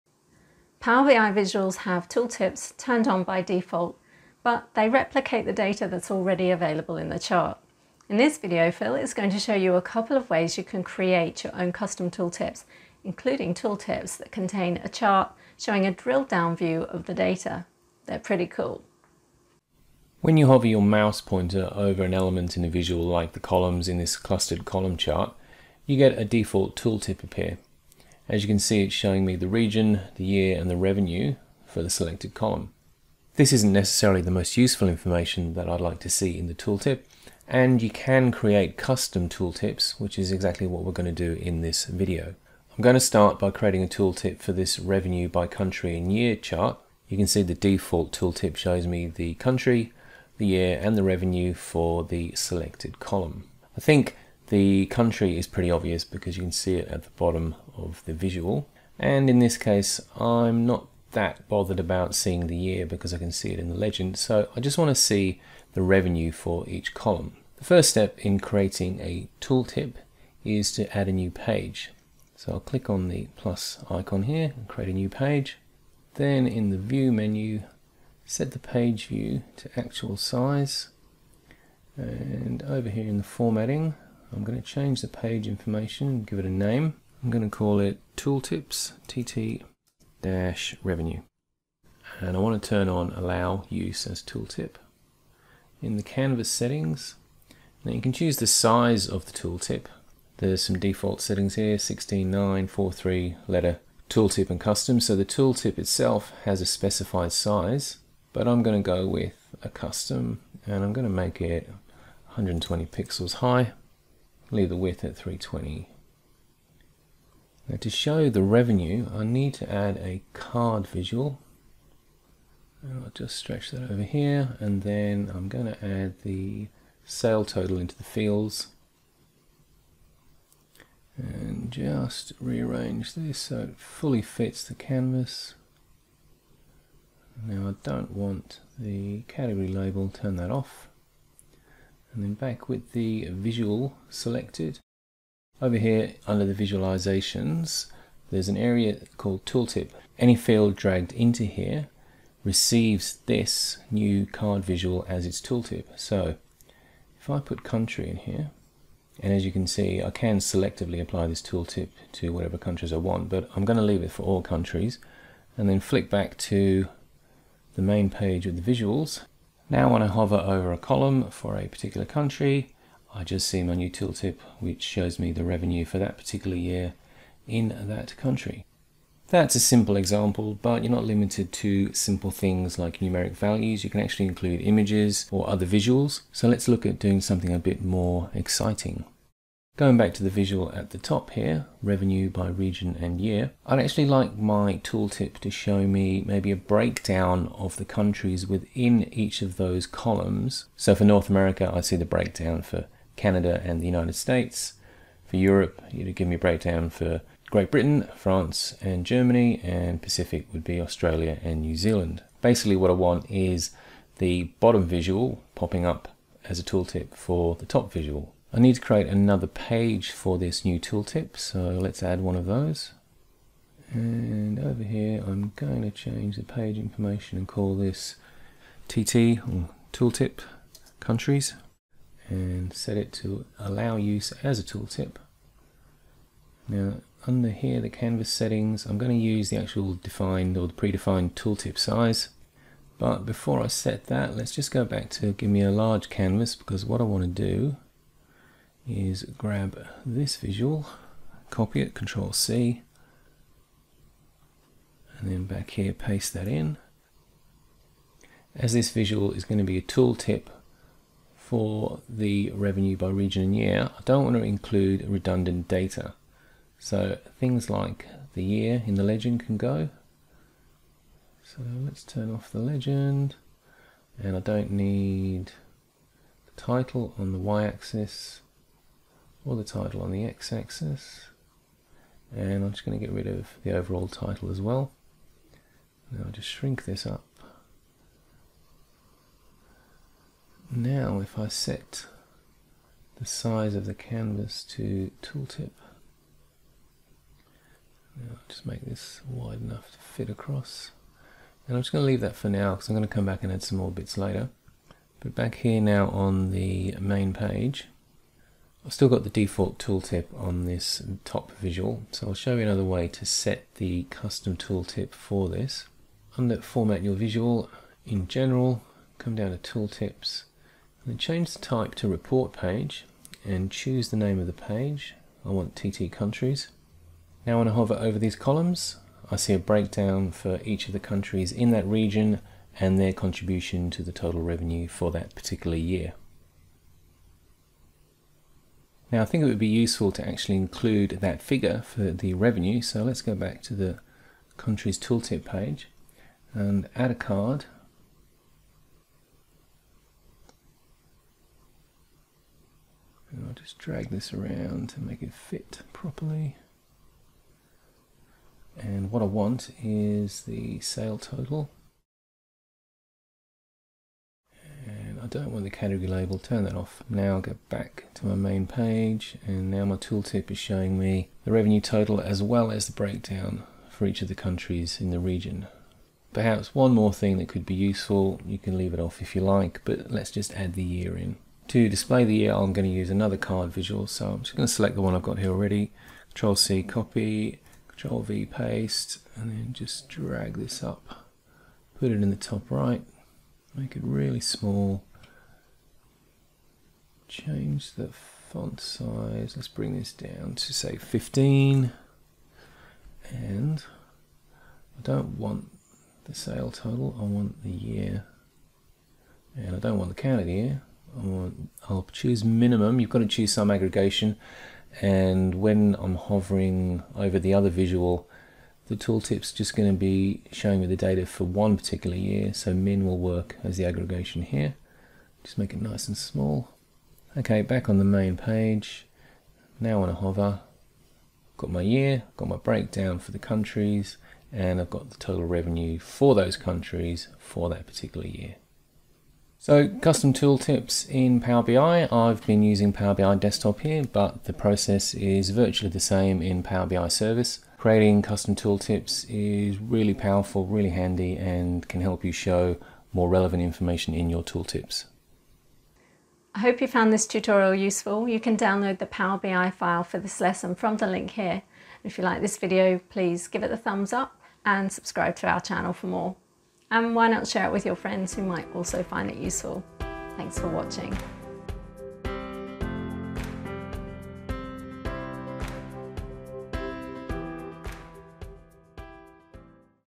Cool Tricks with Power BI Custom Tooltips - Embedded Visuals and more!
Enhance user experience with visuals embedded inside tooltips giving users a drilled down view of the data.

0:00 Introduction
1:07 Creating a custom tooltip
3:09 Chart/visual inside a tooltip